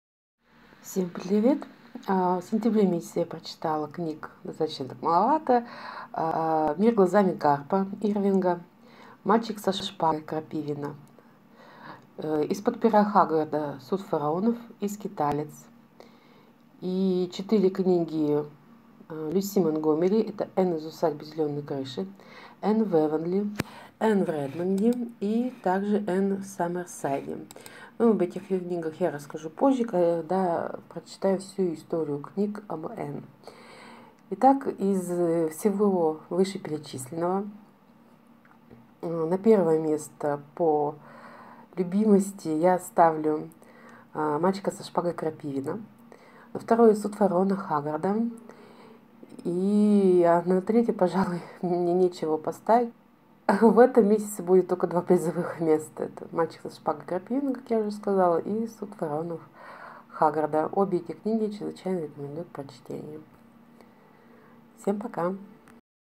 Список прочитанного за сентябрь 2017
В видео упомянуты следующие книги:
А так же книги из серии "Истории об Энн" Л.Монтгомери